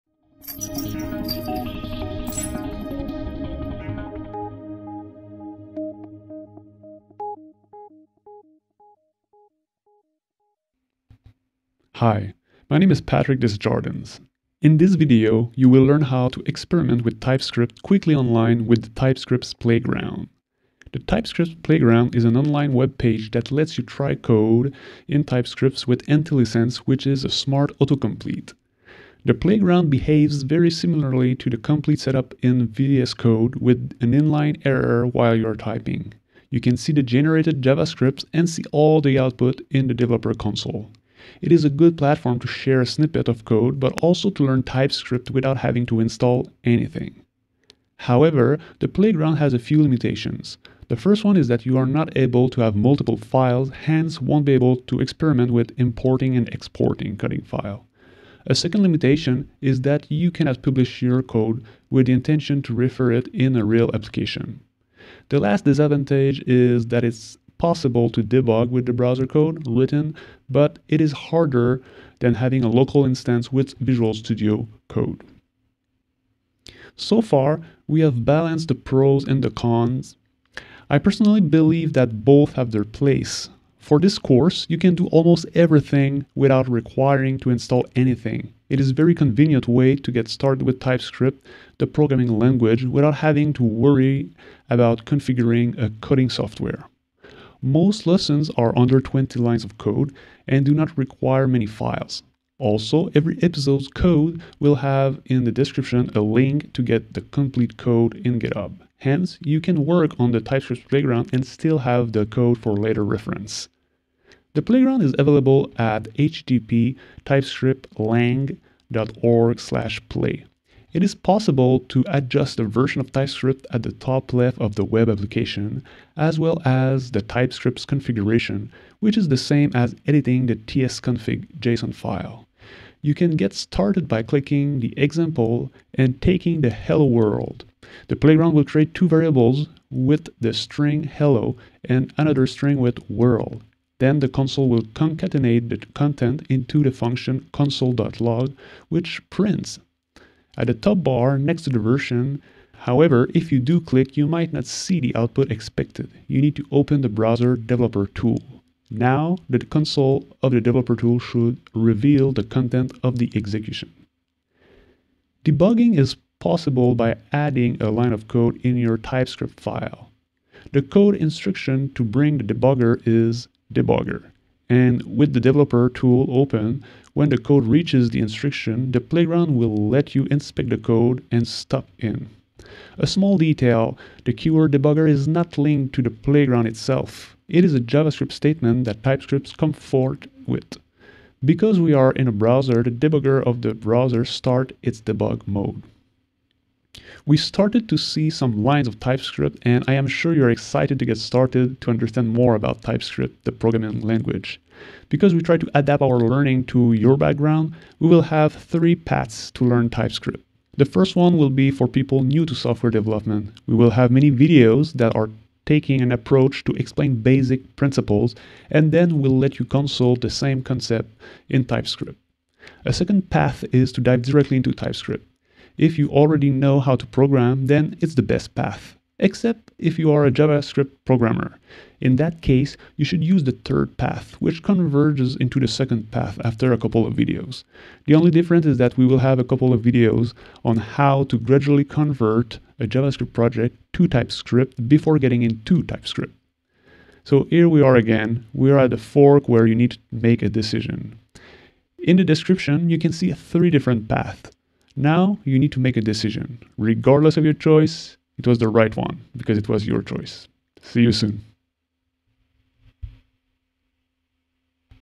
TypeScript’s playground - Typescript Unchained - Episode 013
You will learn how to experiment with TypeScript quickly online with TypeScript’s playground.

📖Chapters
0:20 Pros of using the TypeScript Playground
0:50 Const of using the TypeScript Playground
2:00 TypeScript Playground Configuration
2:45 Generation Script

🚏Paths
1) New Programming
3) From JavaScript

📝Source Code
N/A